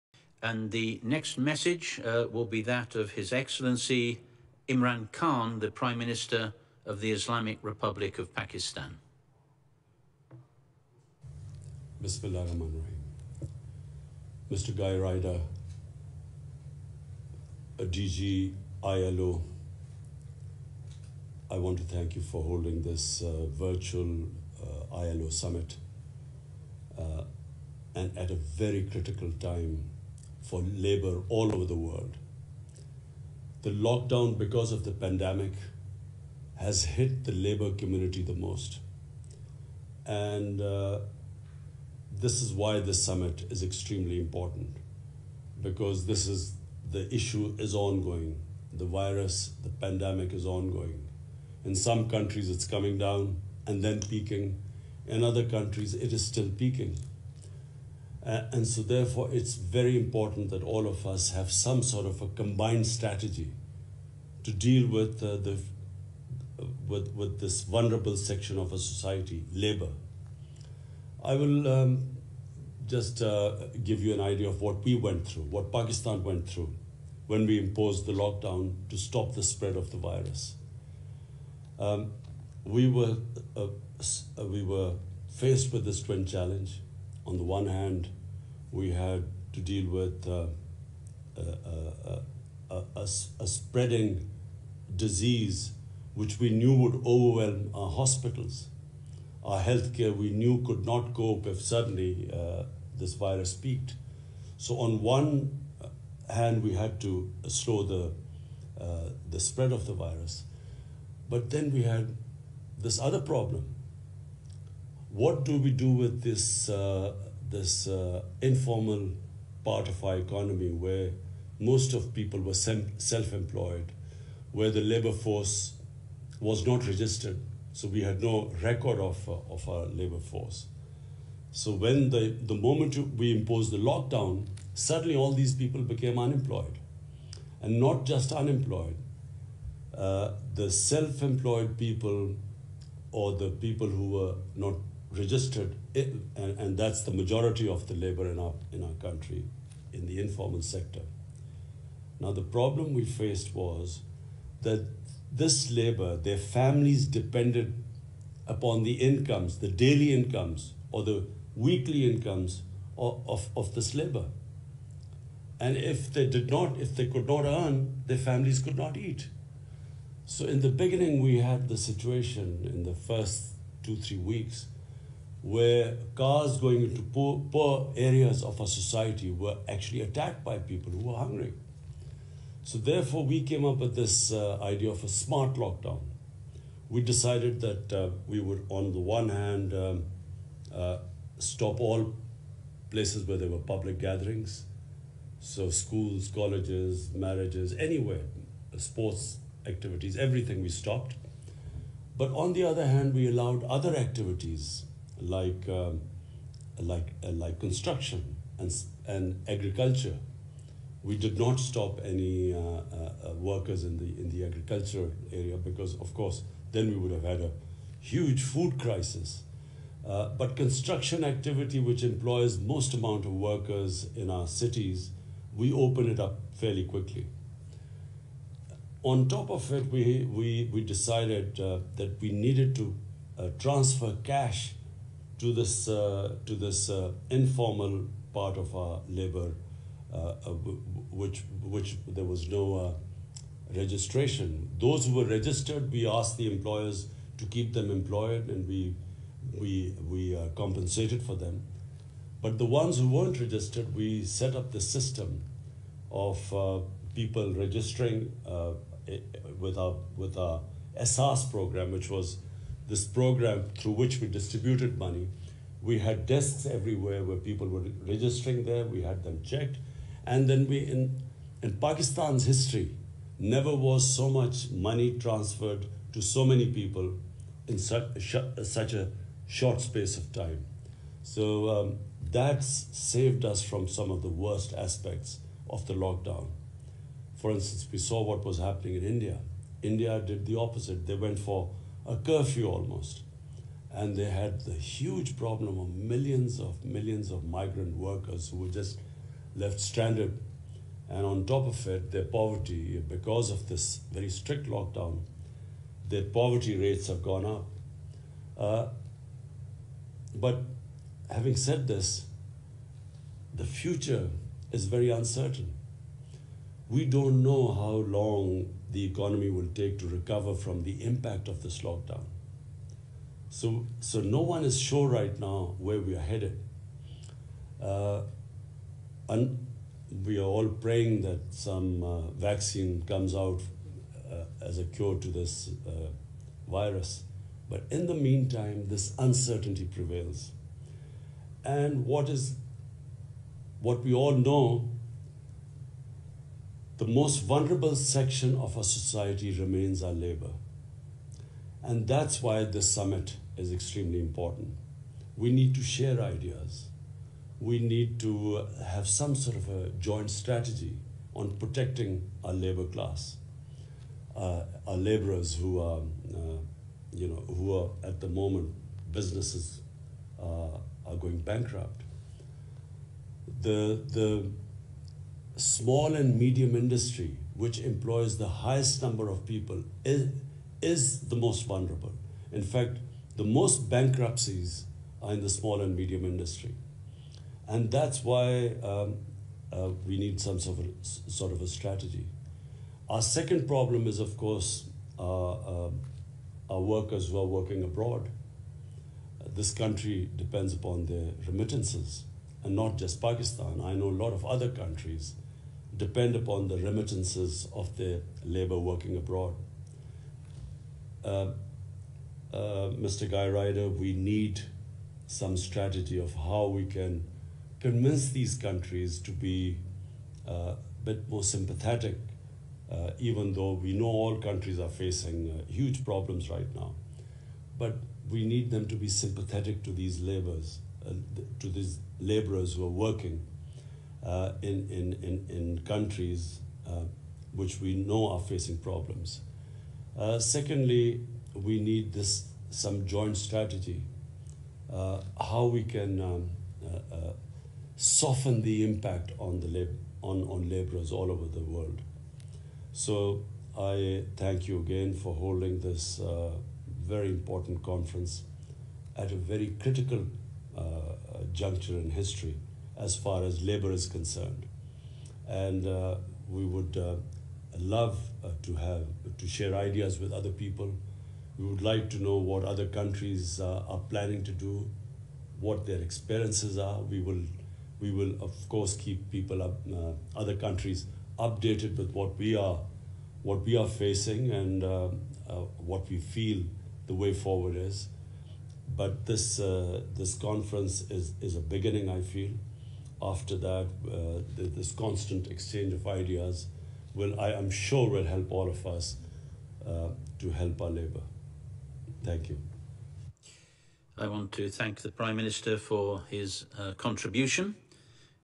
Prime Minister Imran Khan Speech At ILO Global Summit On COVID-19 | PMO Pakistan | 8 July 2020
Prime Minister Imran Khan has urged the world community to formulate a combined strategy to protect the vulnerable segments of the society especially the laborers from the impacts of COVID-19.

He was addressing an online ILO Global Summit on COVID-19 and the World of Work.

The Prime Minister pointed out that many countries including Pakistan depend upon remittances sent by the overseas workers. He said though the host countries are also faced with many problems due to Coronavirus, yet we need to convince them to be more sympathetic to these laborers.

Imran Khan emphasized that the lockdown has hit the labor community the most. He said the future is very uncertain as we do not know how long the economy will take to recover from the impacts of the pandemic. He said we are praying that vaccines come out as a cure to the virus but in the meantime we need to share our experiences and exchange ideas to protect our vulnerable groups.

Sharing the initial lockdown experience of Pakistan, the Prime Minister said it rendered our daily wage workers and laborers unemployed. Hence, he said we came up with the smart lockdown idea under which public gatherings were stopped but we allowed other activities like reopening of construction and agriculture sectors.

Imran Khan said Pakistani government provided cash assistance to the laborers under Ehsaas Program after completing their registration process.  He said it was the first time in Pakistan's history that such a huge money was disbursed in such a short space of time. He said this saved us from the worst effects of the lockdown.